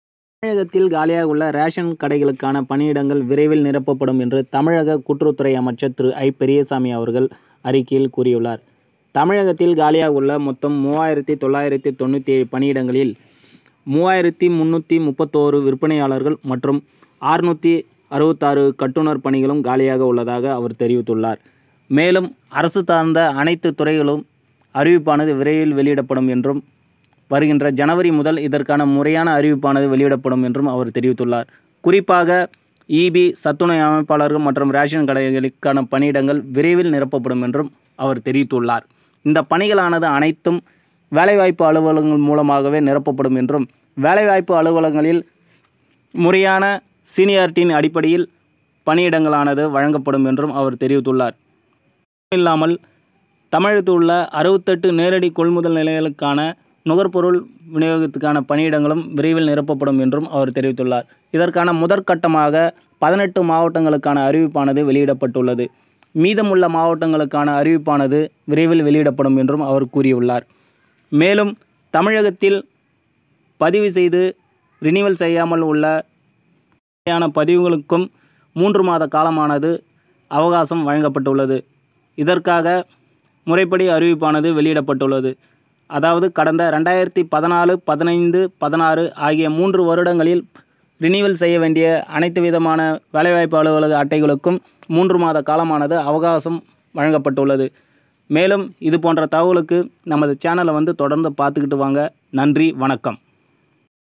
அனைத்து மாவட்ட ரேஷன் கடையில் 3997 காலிபணியிடங்கள் அறிவிப்பு | வேலை நிரப்பபடும் புதிய முறை
அனைத்து மாவட்ட ரேஷன் கடையில் 3997 காலிபணியிடங்கள் அறிவிப்பு | வேலை நிரப்பபடும் புதிய முறை | Renewal